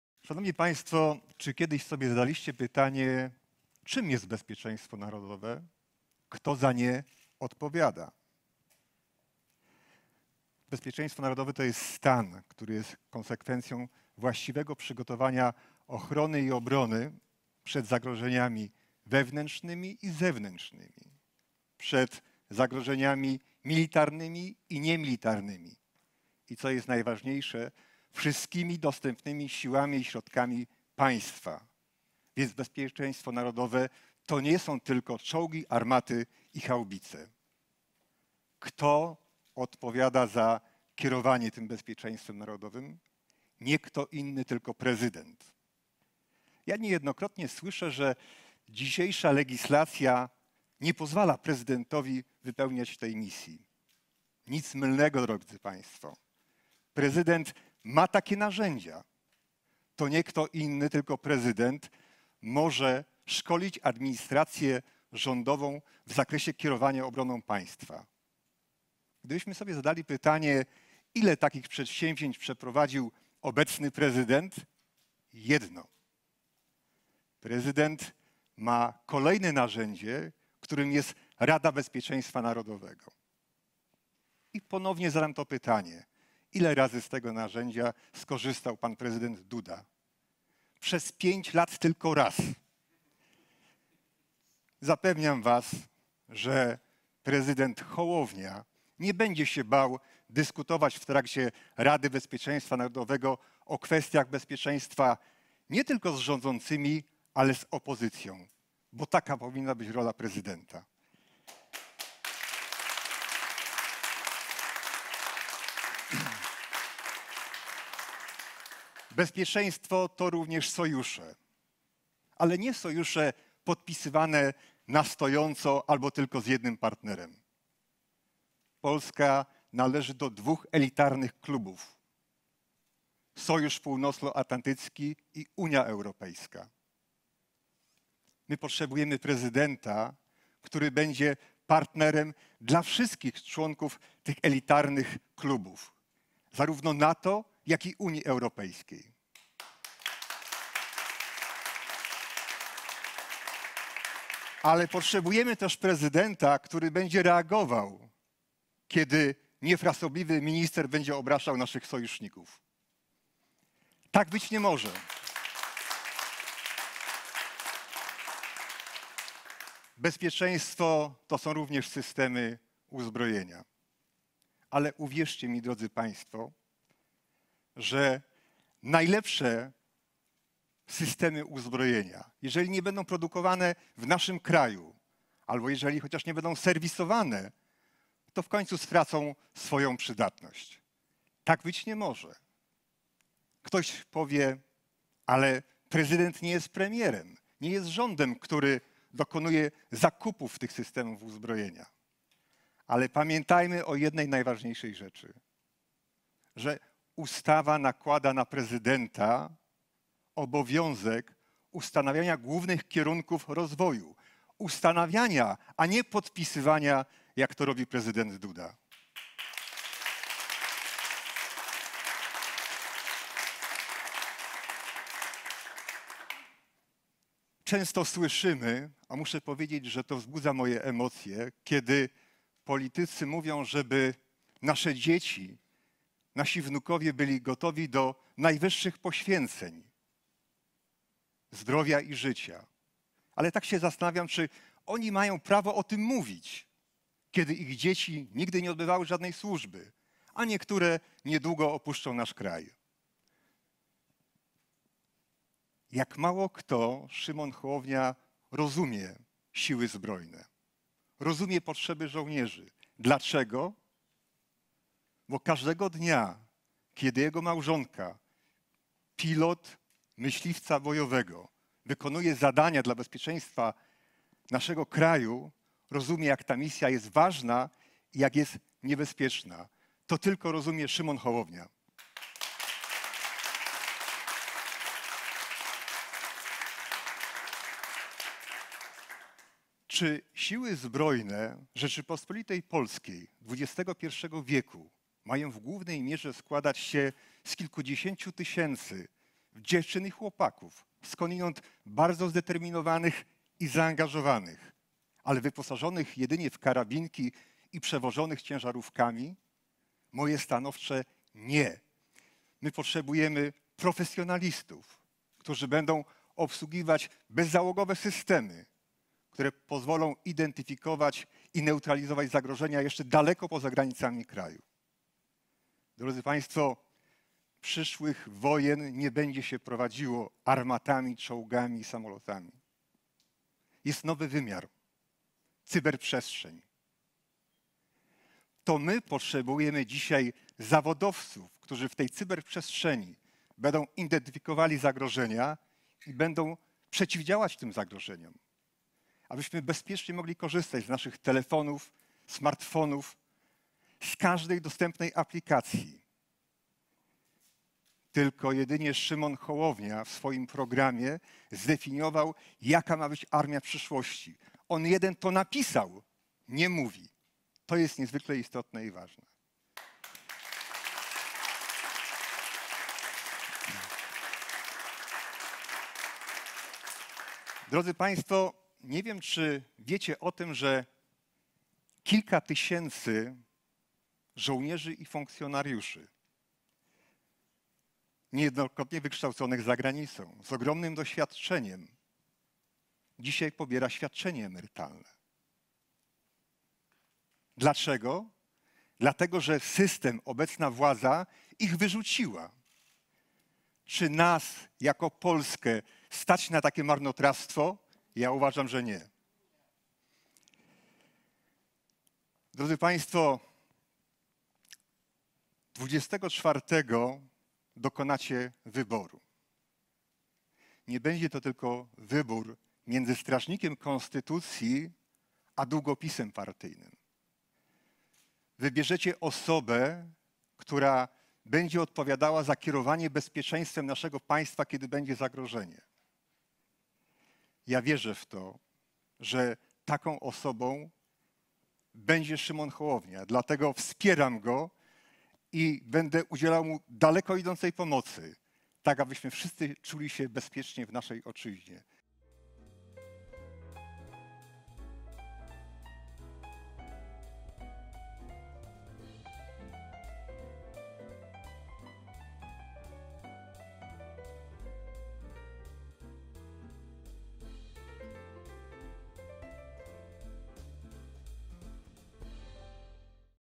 Pokolenie nie kadencja cz.I - gen. M. Różański, doradca ds. bezpieczeństwa
Szymon Hołownia wraz z ekspertami ze swojego sztabu przedstawia czołowe zasady swojej kampanii i prezydentury.
Linki:
Tu możesz wpłacić na kampanię prezydencką
Odbiorca: Komitet Wyborczy Kandydata na Prezydenta RP Szymona Hołowni